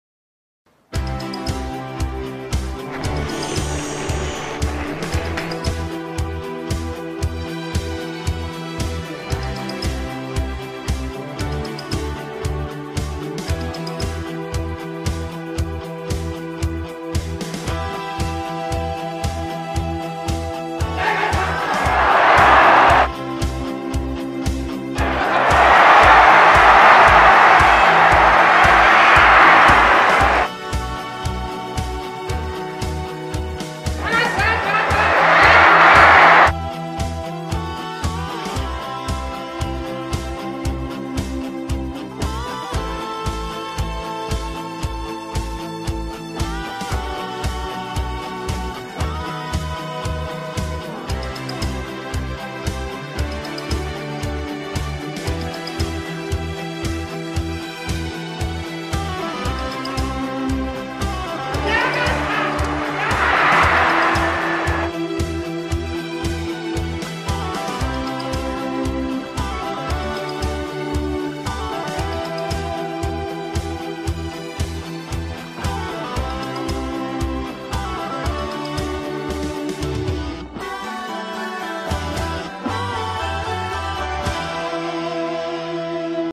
昭和39年9月場所大相撲エンディング
昭和39年9月場所の大相撲エンディングです。
先場所名古屋場所も今後公開していきます。
大鵬が3場所ぶりの優勝、今年3回目の優勝を決めました。
同じく休場明けの柏戸は5日目に肩を痛め休場。
もう1人の横綱栃ノ海も9勝と不調でした。
その中で、豊山、北葉山、佐田の山の3大関が勝ち星を重ね、優勝争いを展開します。ただ、最終的には大鵬に3力士全員が敗れ、独走を許しました。